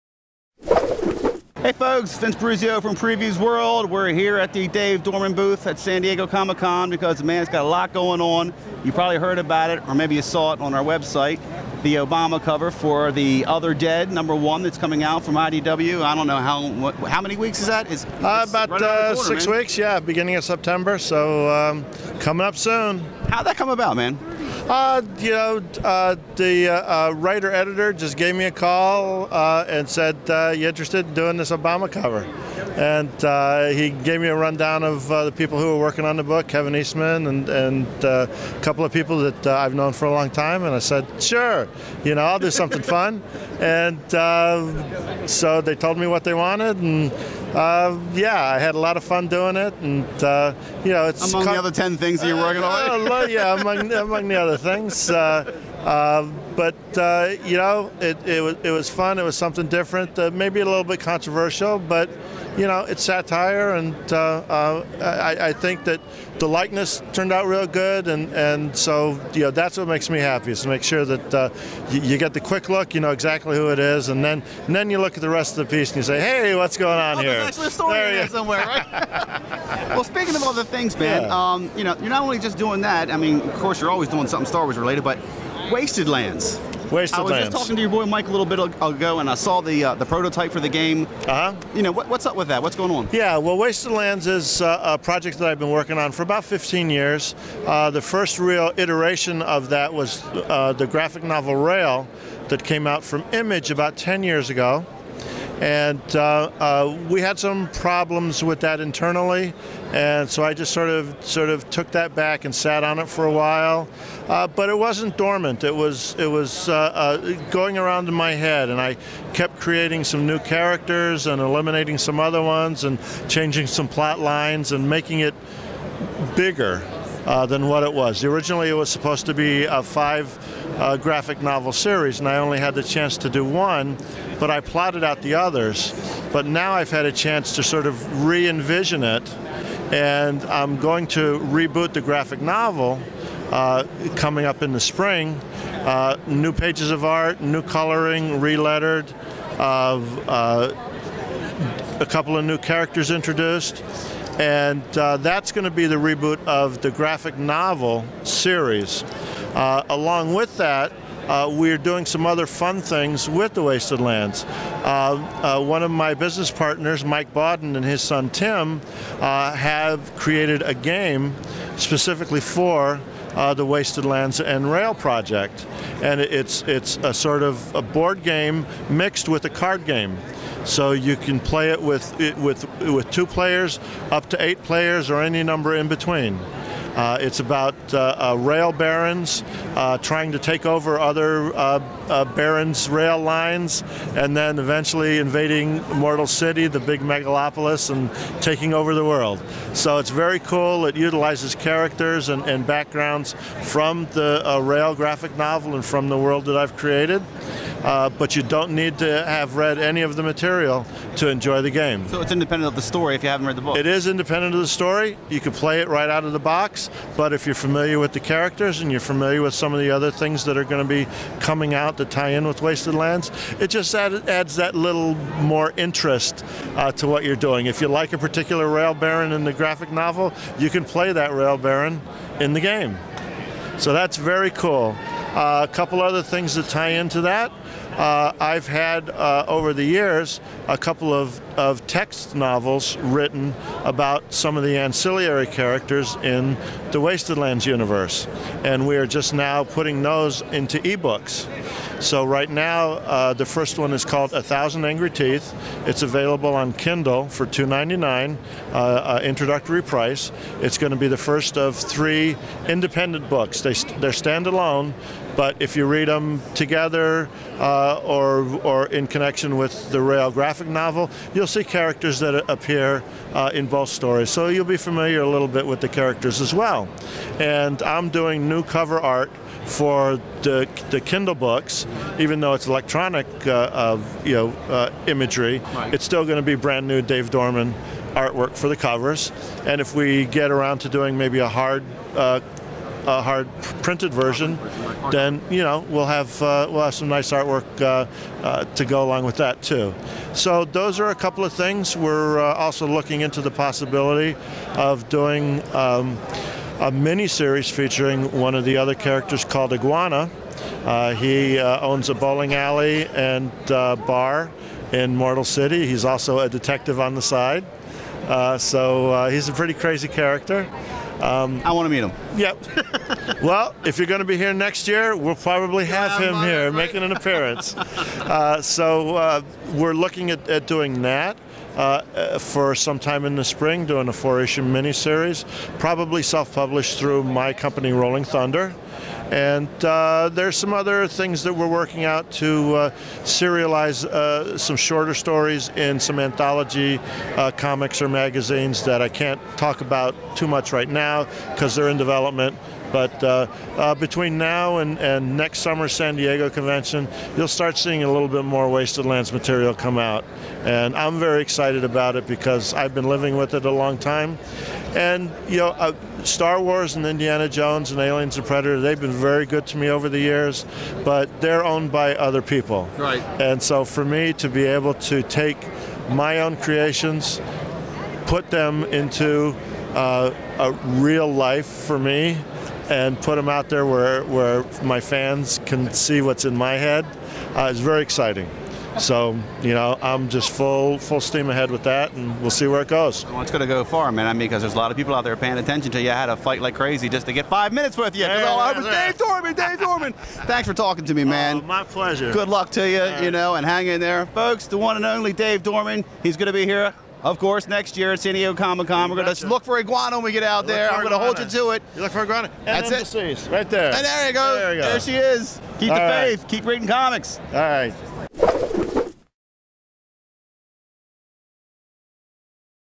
Dave Dorman Talks About Wasted Lands
Award-Winning Artist Dave Dorman talks with PREVIEWSworld's Vince Brusio about his plans for Wasted Lands and his President Obama cover for IDW's The Other Dead.